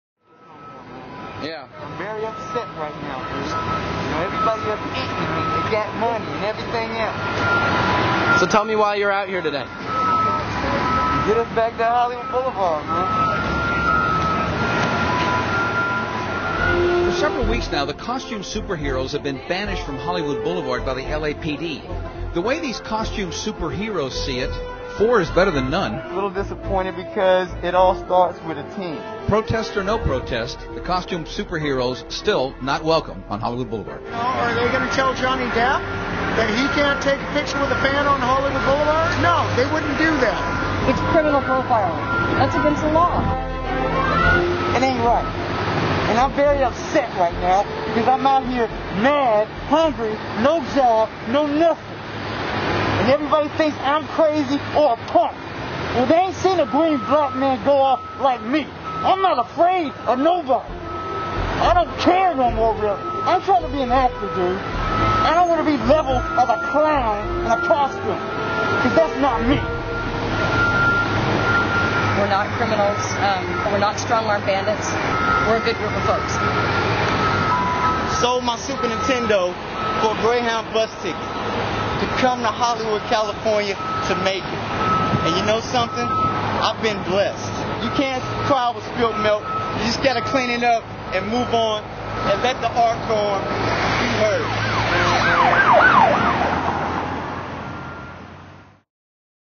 Heroes be Heard (Notorious Double Descent)
The costume characters of Hollywood Blvd were recently arrested and banished from the streets. Superman, Darth Vadar, White Power Ranger and The Hulk take their fight to City Hall.